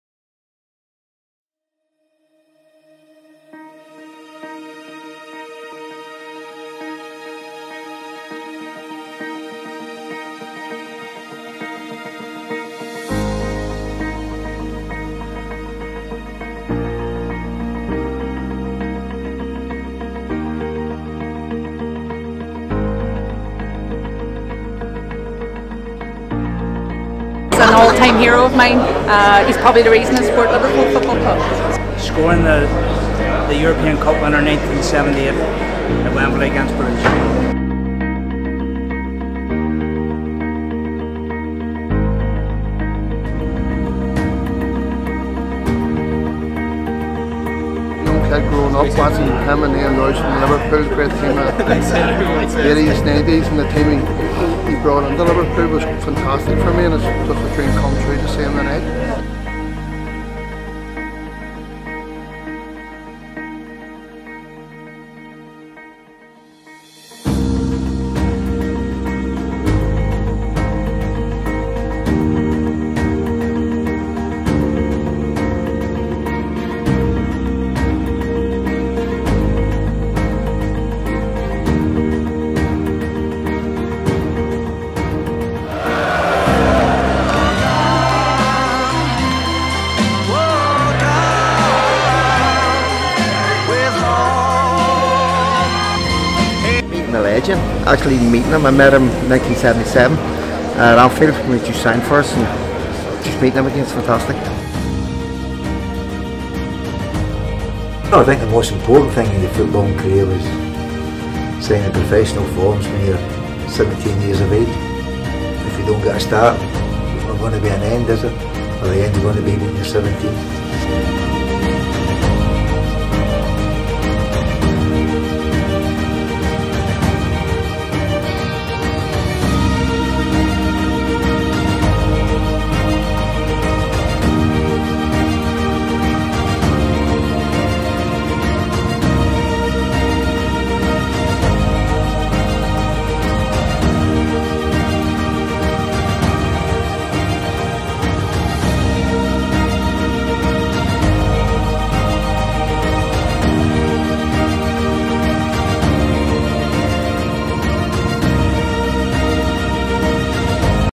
An Evening with Sir Kenny Dalglish MBE
All Things Management were invited to Belfast to film ‘An Evening with Kenny Dalglish’ in January 2020. Our team flew out to Belfast to capture what was an incredible evening for Kenny Dalglish and Liverpool FC super fans.

For the first time live, Kenny Dalglish talked about his life in and out of football. In a question and answer format, he discussed the great times and the tough times he has endured throughout his illustrious career.

Kenny was interviewed by his daughter, Kelly Cates, now a successful Sky Sports pundit. There was also a chance for fans to meet Kenny in person on the night and get a professional photo with the King himself and All Things Management were there to capture it all.